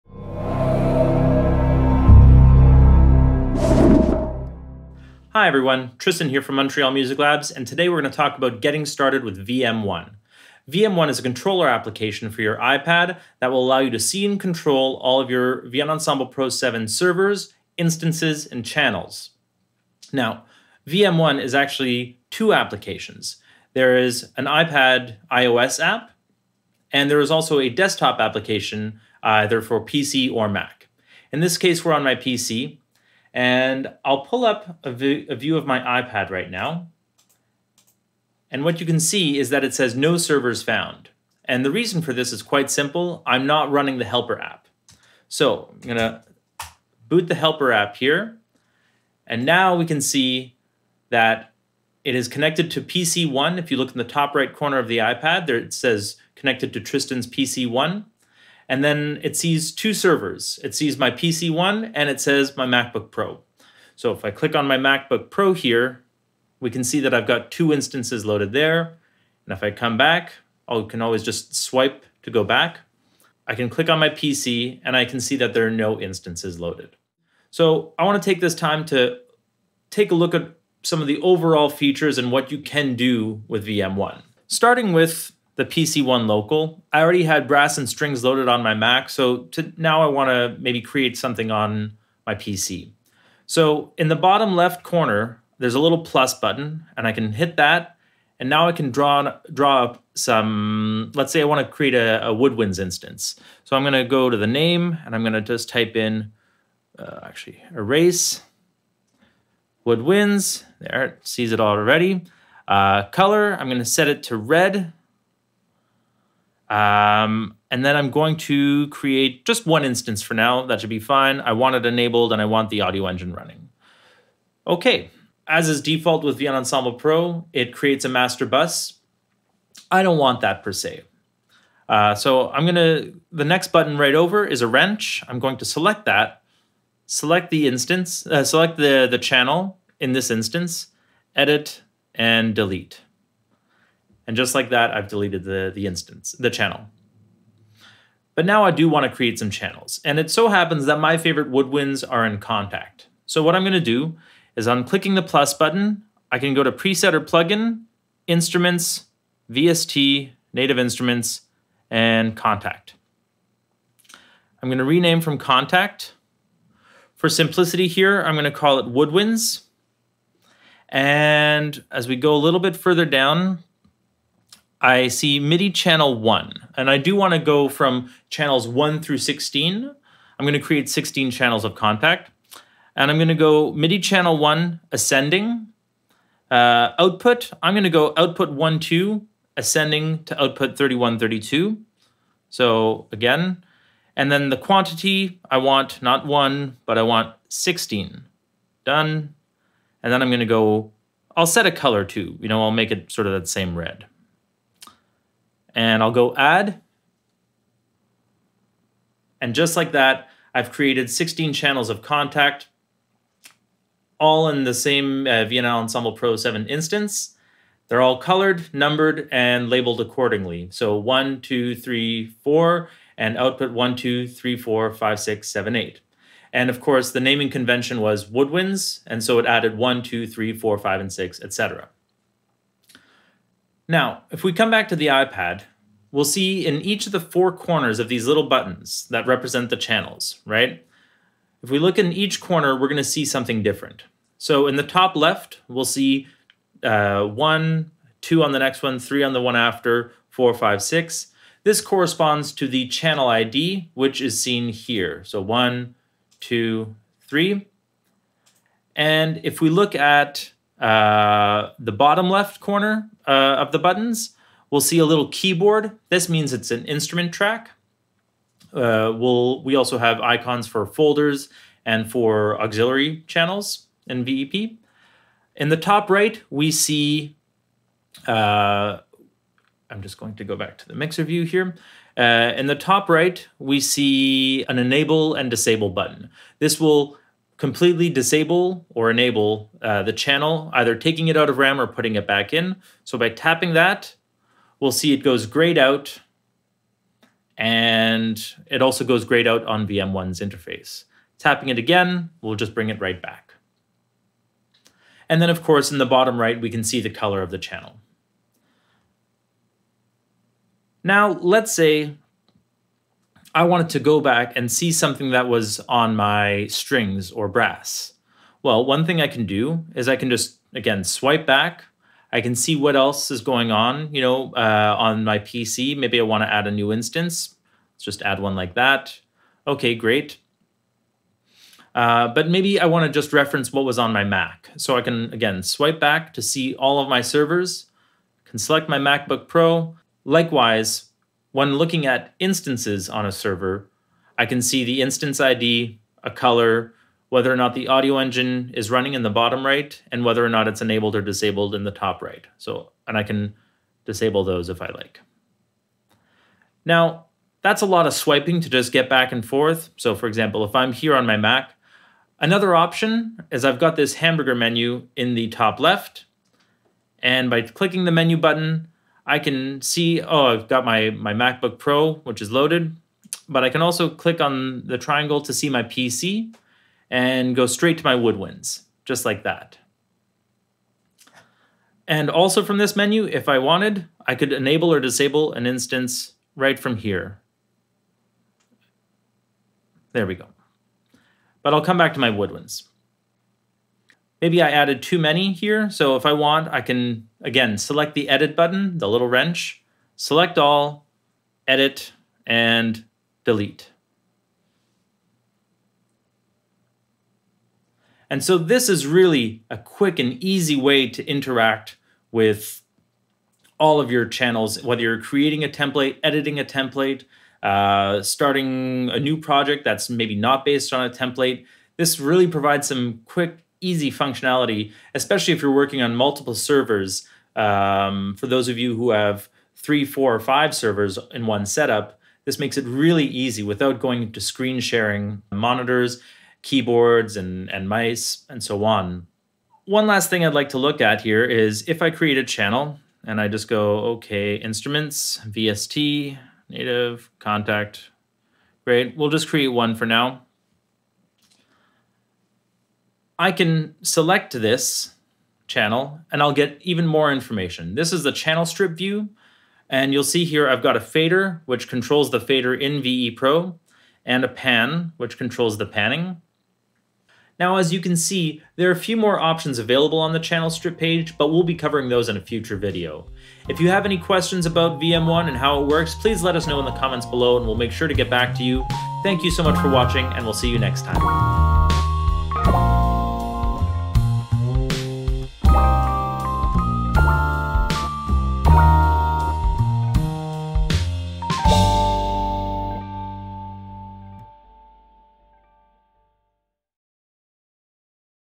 VM1 - Getting Started - Part 1
An in-depth walk through of the primary features of VM1 and how to set it up.

VM1 is an iPad controller application for Vienna Ensemble Pro 7. VE Pro 7 is a powerful tool with a rich feature set and VM1 is designed to harness that power in a quicker and easier way than ever before!  Moreover, VM1 allows for new ways of interacting with VE Pro 7 to enhance your creative expression.